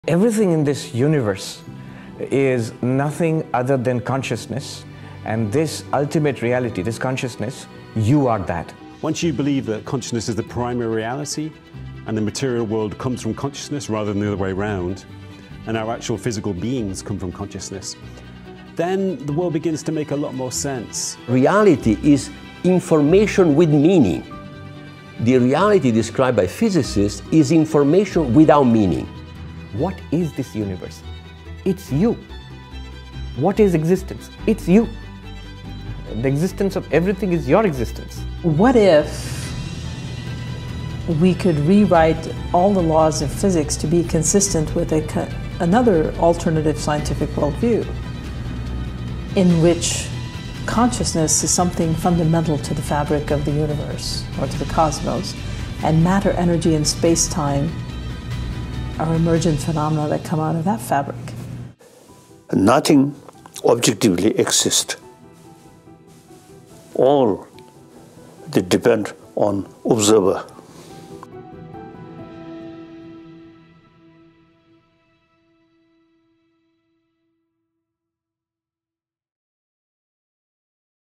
You Are the Universe: Consciousness, Reality & the New Physics
What if everything you’ve been told about reality is wrong?

In this mind-expanding journey inspired by Quantum Convergance, top scientists, spiritual leaders, and visionaries come together to explore a radical new paradigm — where consciousness is not a byproduct of the brain, but the fundamental substance of reality itself.

🧬 Is the material world an illusion?
🌌 Can we rewrite the laws of physics to align with consciousness?
👁️ What if you are the universe observing itself?

🎙️ Topics include:
– Quantum mechanics and the observer effect
– Vedanta, Buddhism & ancient wisdom
– The hard problem of consciousness
– How matter, energy, and space-time may emerge from mind